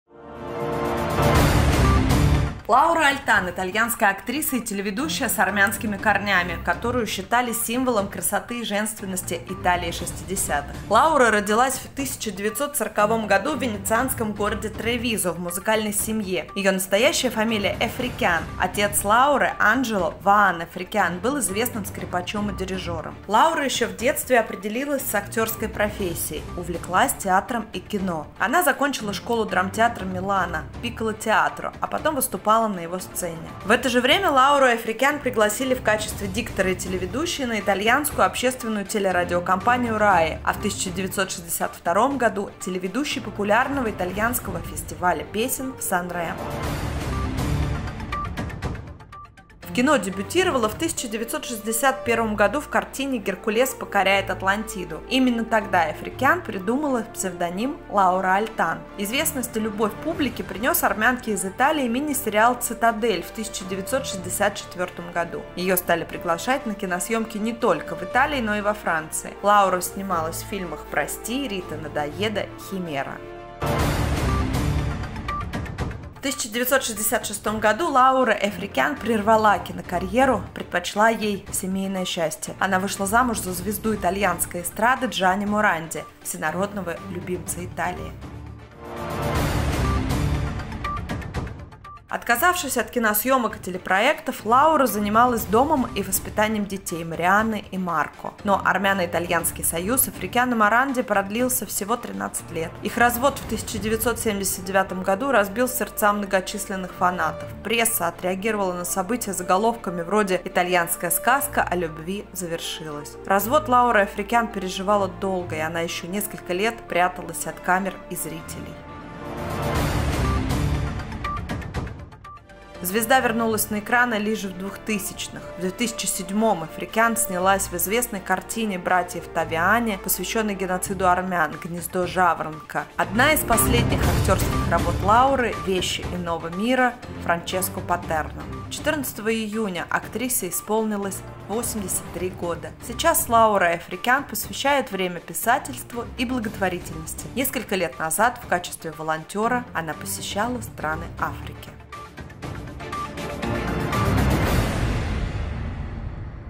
Армянка, ставшая символом красоты Италии и женой Моранди
Лаура Альтан – итальянская актриса и телеведущая с армянскими корнями, которую считают символом красоты и женственности Италии 60-х.

Лаура родилась в 1940 году в венецианском городе Тревизо в музыкальной семье. Ее настоящая фамилия – Эфрикян. Отец Лауры — Анжело (Ваан) Эфрикян, был известным скрипачом и дирижером.

Лаура еще в детстве определилась с актерской профессией, увлекалась театром и кино. Она закончила школу драмтеатра Милана — «Пикколо-театро», а потом выступала на его сцене.

В это же время Лауру Эфрикян пригласили в качестве диктора и телеведущей на итальянскую общественную телерадиокомпанию RAI, а в 1962 году -  телеведущий популярного итальянского фестиваля песен в Сан-Ремо.

В кино дебютировала в 1961 году в картине «Геркулес покоряет Атлантиду». Именно тогда Эфрикян и придумала псевдоним Лаура Альтан.

Известность и любовь публики принес армянке из Италии мини-сериал «Цитадель» в 1964 году. Ее стали приглашать на киносъёмки не только в Италии, но и во Франции.

Лаура снималась в фильмах «Прости», «Рита-надоеда», «Химера».

В 1966 году Лаура Эфрикян прервала карьеру – предпочла ей семейное счастье. Она вышла замуж за звезду итальянской эстрады Джанни Моранди, всенародного любимца Италии.

Отказавшись от киносъемок и телепроектов, Лаура занималась домом и воспитанием детей —  Марианны и Марко.

Но армяно-итальянский союз Эфрикян и Моранди продлился всего 13 лет. Их развод в 1979 году разбил сердца многочисленных фанатов. Пресса отреагировала на событие заголовками вроде «итальянская сказка о любви завершилась».

Развод Лаура Эфрикян переживала долго, и она еще на два десятилетия спряталась от камер и зрителей.
Звезда вернулась на экраны лишь в 2000-ых.

В 2007 году Эфрикян она снялась в известной кинокартине братьев Тавиани, посвященной Геноциду армян, «Гнездо жаворонка».

Она из последних актерских работ Лауры – «Вещи иного мира» Франческо Патерно.
14 июня актрисе исполнилось 83 года.

Сейчас Лаура Эфрикян посвящает время писательству и благотворительности. Несколько лет назад в качестве волонтера посещала страны Африки.